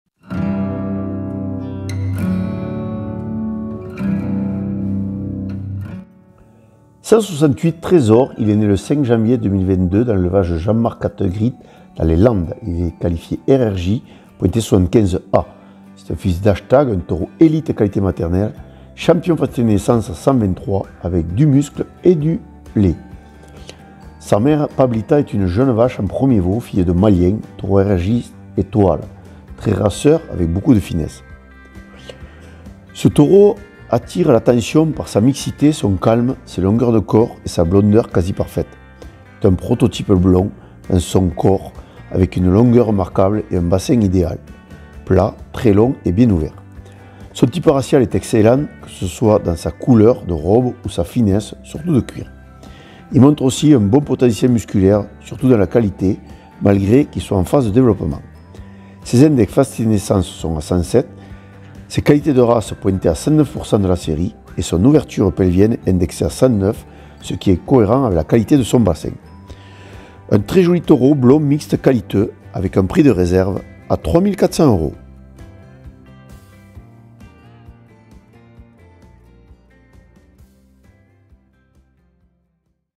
SR168 - TRESOR - RRJ
Fils de HASHTAG x PABLITA (MALIEN), TRESOR appartient à M. CANTEGRIT JEAN-MARC (40), il est RRJ